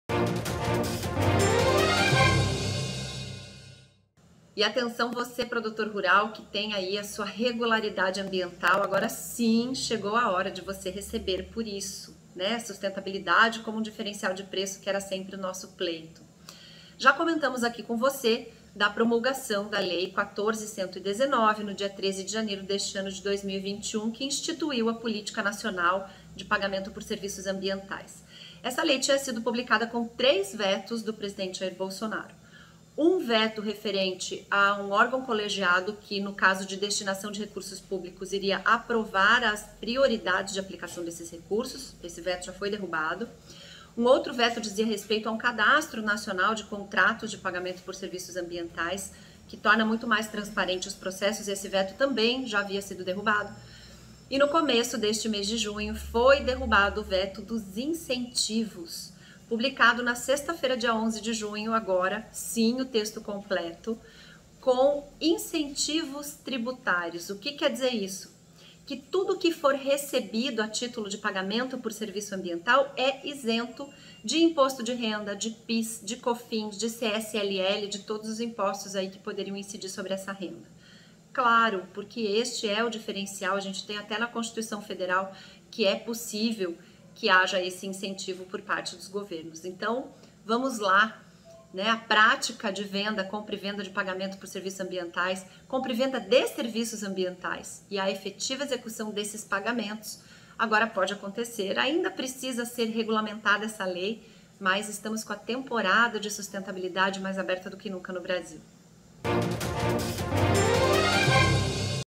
Direito e Certo: Pagamentos por Serviços Ambientais contarão com isenção tributária
A isenção tributária para receitas geradas a partir de pagamentos por serviços ambientais é o assunto da coluna Direito e Certo de hoje, com a advogada Samanta Pineda.